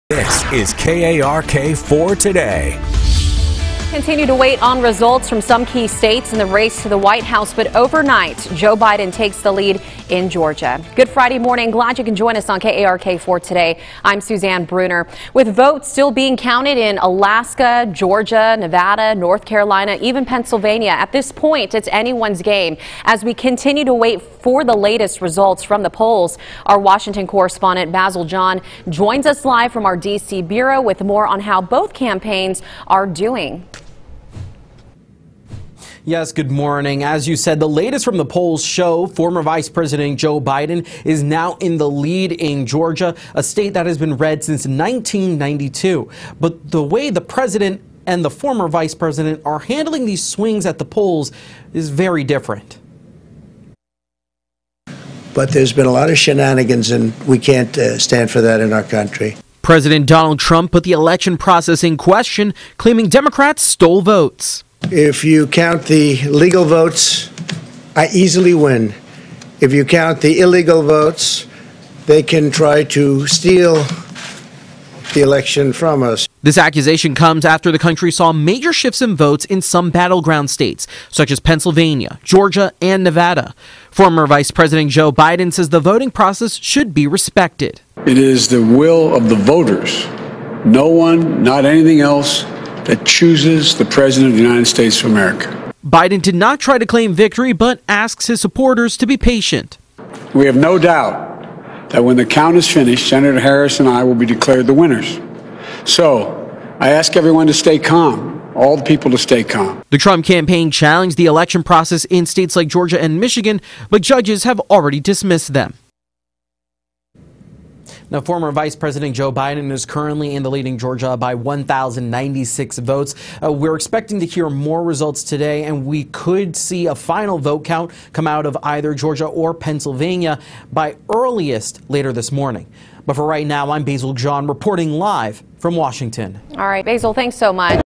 Will we find out Friday who the next president is?
With no clear winner on Friday morning, all eyes will be on Pennsylvania, Georgia and Nevada to see if they release enough results for a winner to be projected in the presidential race.